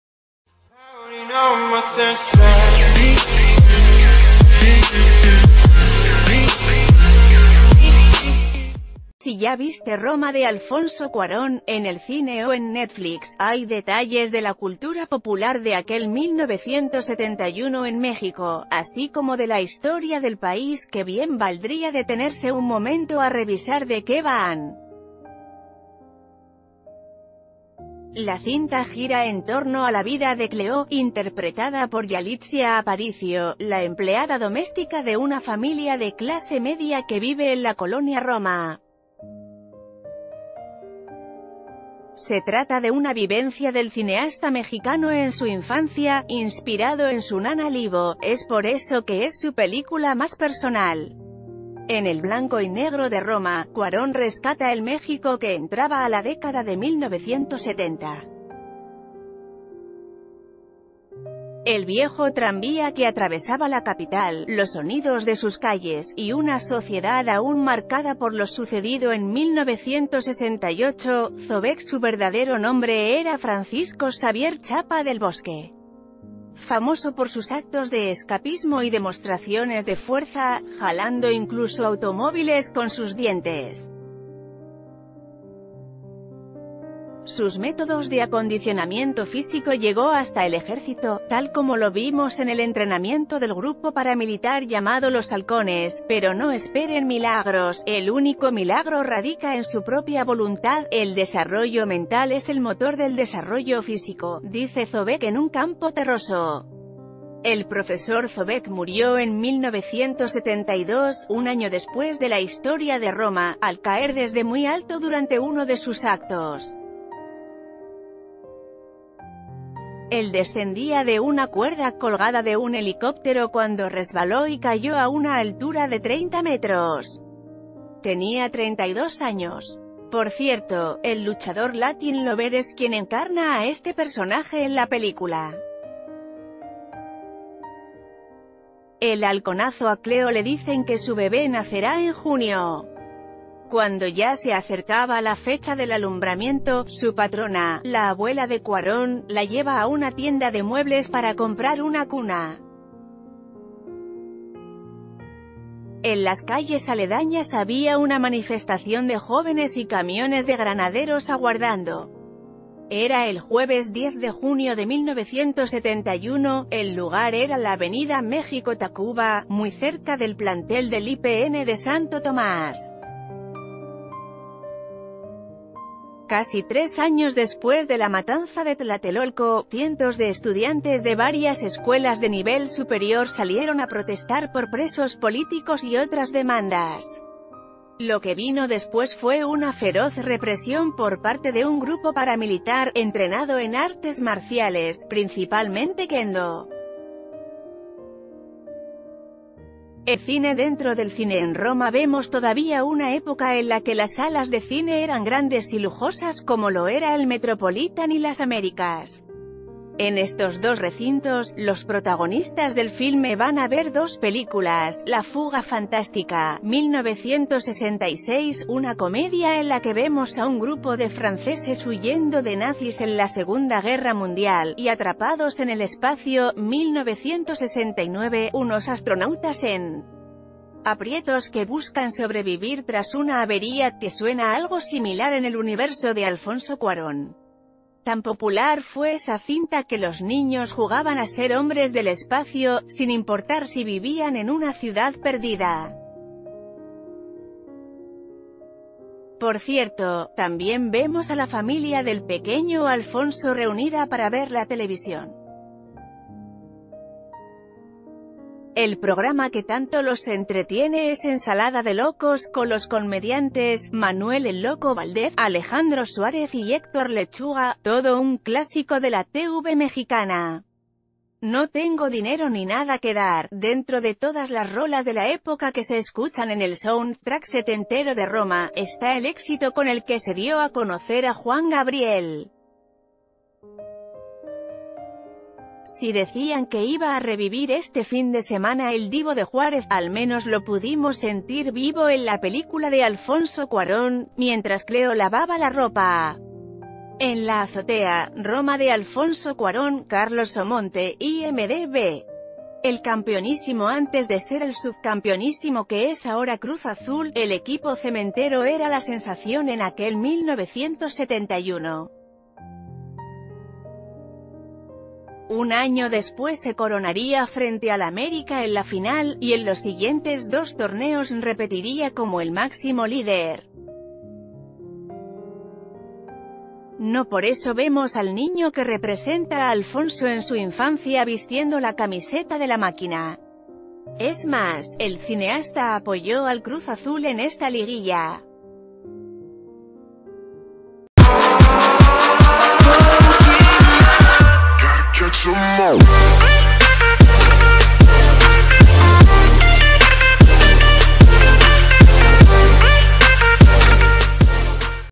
De Zobek al Cruz Azul, la cultura pop setentera en 'Roma'
Si ya viste Roma de Alfonso Cuarón, en el cine o en Netflix, hay detalles de la cultura popular de aquel 1971 en México, así como de la historia del país, que bien valdría detenerse un momento a revisar de qué van. La cinta gira entorno a la vida de Cleo, interpretada por Yalitzia Aparicio, la emple...